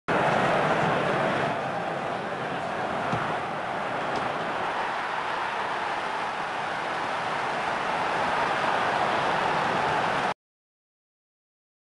Getafe CF 2 - 1 Sevilla FC
Perotti accorcia le distanze